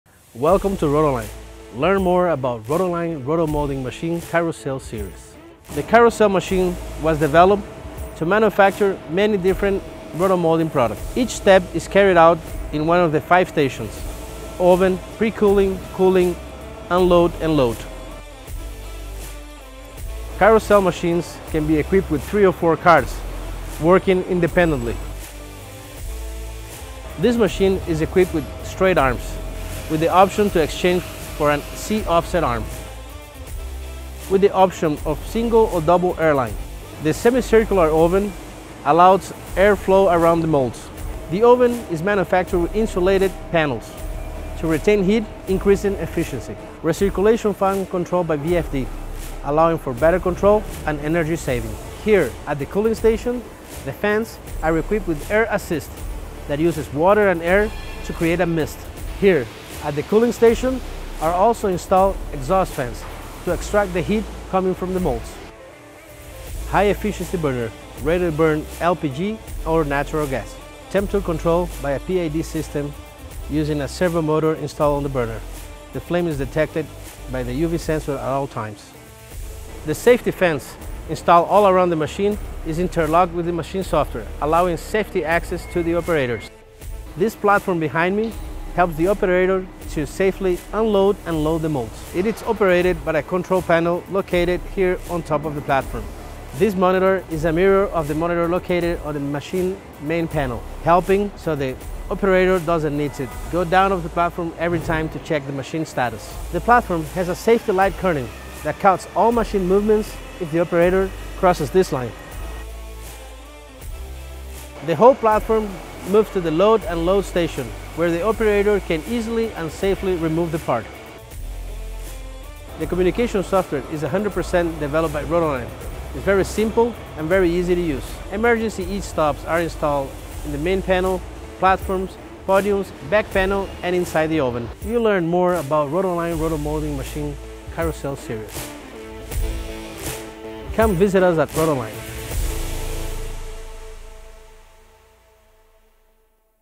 Rotoline Rotomolding Machine - Carrossel Series
With five independent stations, the oven; pre-cooling; cooling; loading/unloading and stand-by steps, can be made individually, reducing machine downtime, as each arm can be in at a different station than the others.